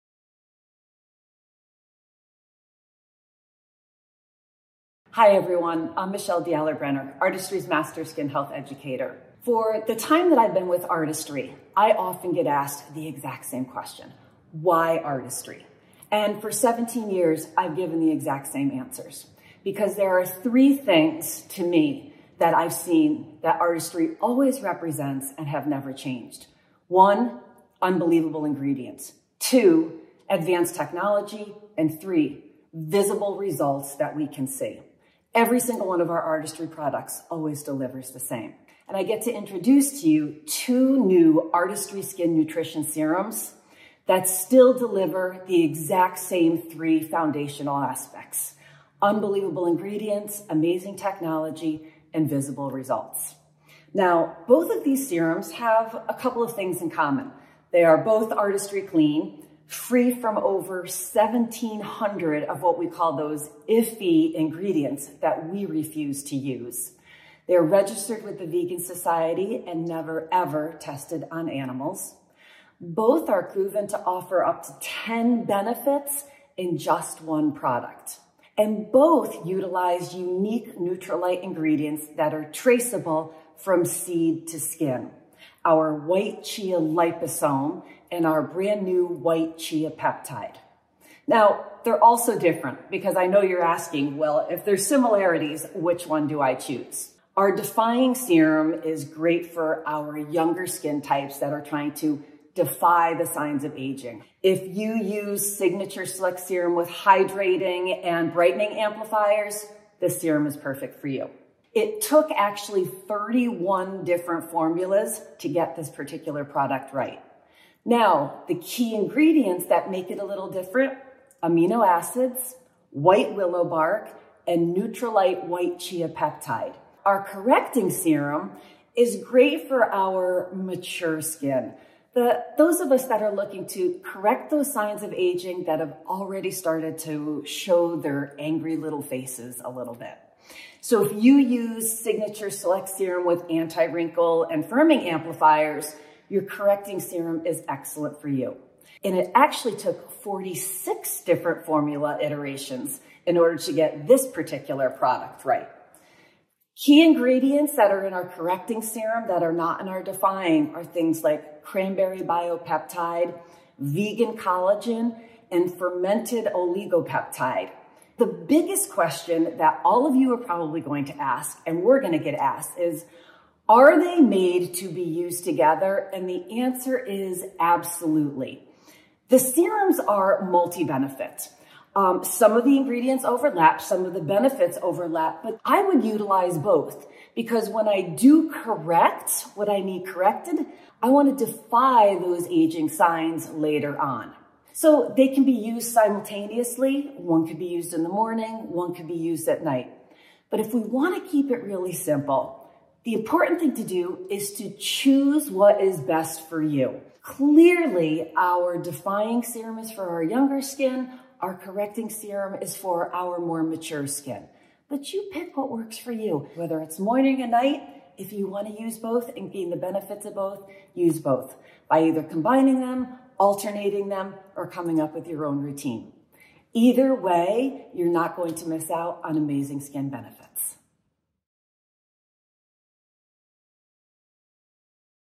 ¿Qué suero Skin Nutrition es mejor para mí?
La experta en cuidado de la piel Michelle D'Allaird-Brenner habla sobre los Sueros desafiante y corrector Artistry Skin Nutrition. Aprende las diferencias y comprende cómo estos sueros pueden mejorar tu rutina de cuidado de la piel.